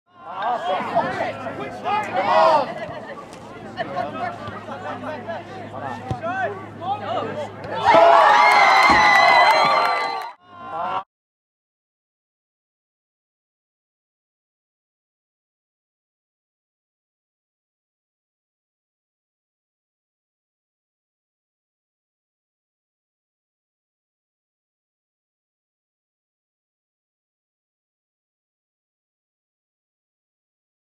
Clark with the sweet upper 90
Clark scores the first goal of the game in the sub-state game vs. West Morgan